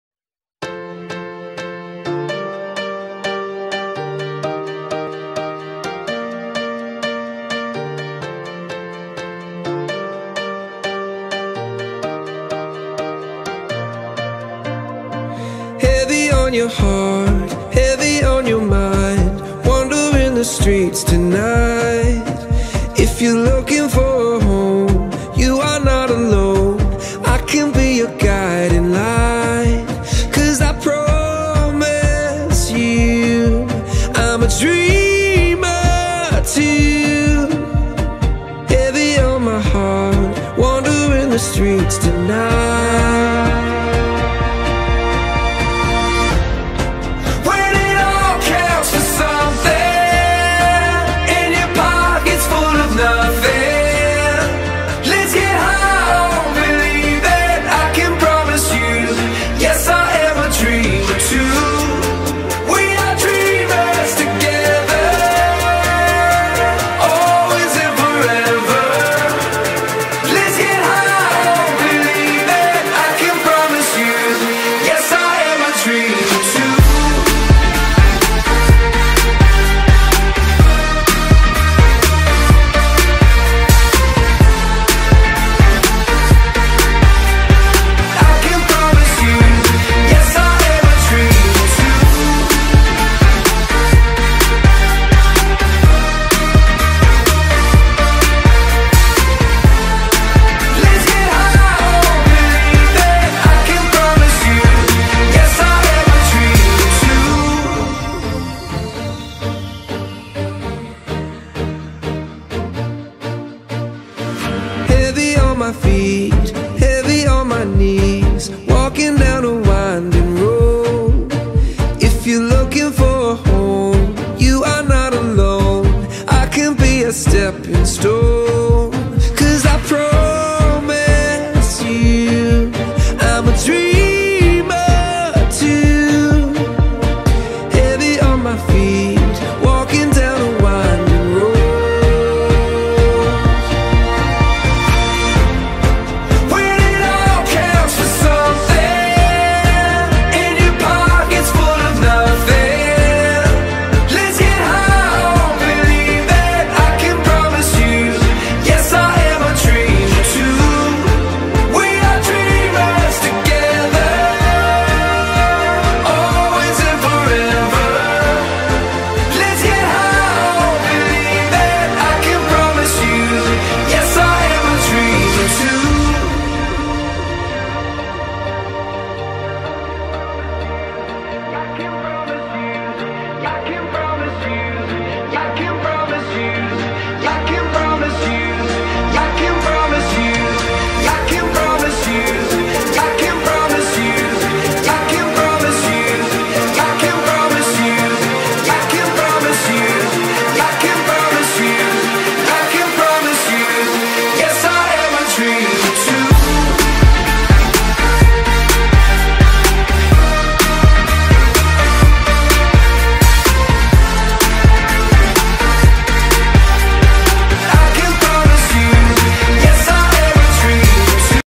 Goals,Assist en Skills van de nieuwe aanwinst Emil Hansson!
Goals, Assist en Skills van onze nieuwe aanwinst Emil Hansson!